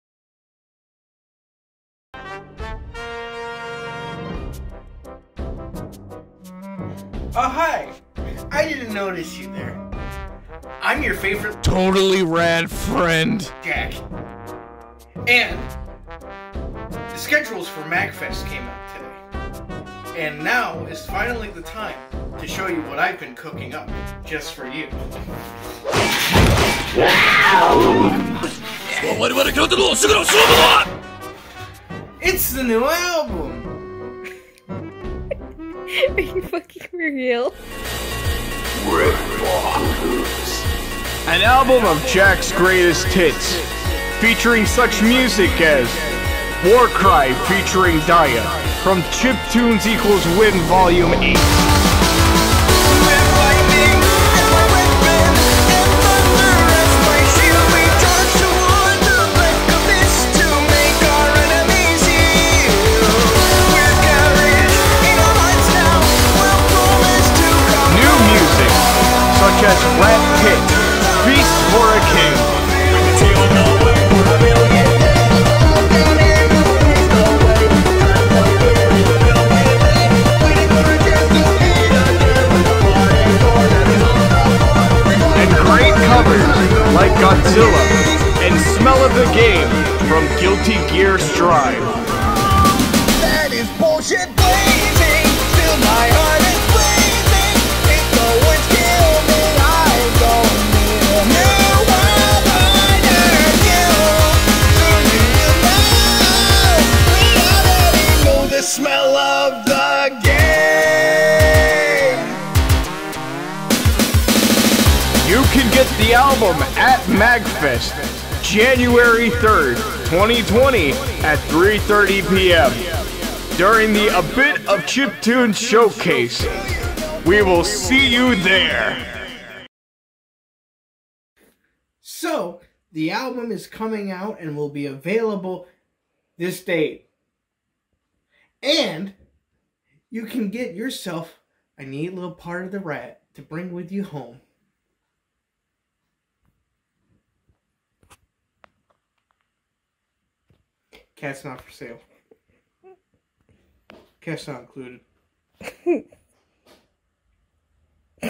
A Very Special Announcement
What could it mean?

MAGFest (aka Music and Gaming Festival) had asked me to perform at Chipspace come 2020! And what better way to celebrate, than with an album of some of my greatest hits, along with some new songs!

If you're going to MAGFest, come check me out, and my friends Polaria Poyon and Joe Swensen, Jan. 3rd, 2020, at 3:30 pm EST.